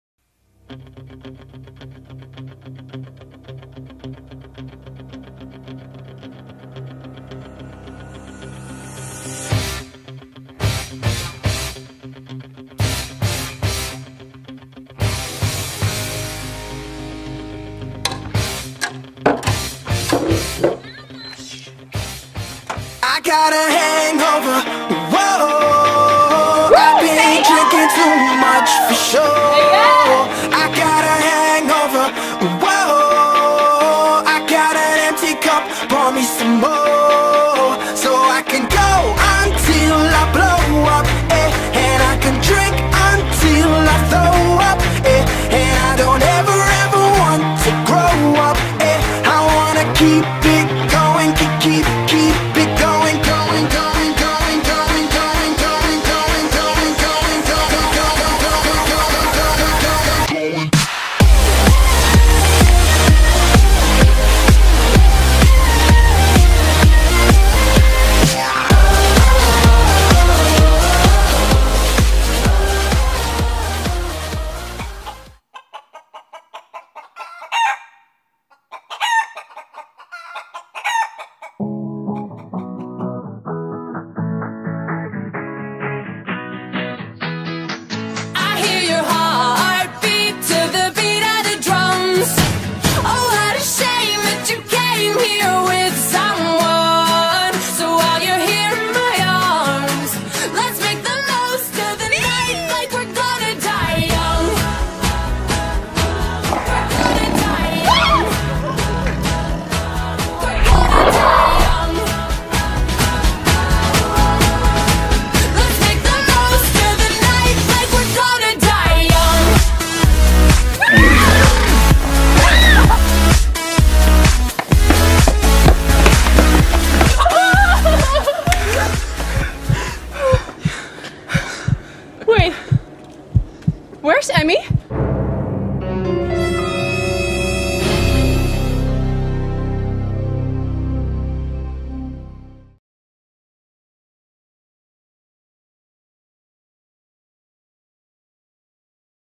The Hangover Trailer
For a school project we had to star in a trailer of a movie. Here is the result! Please tell us what you think about it in the comments below.

2013.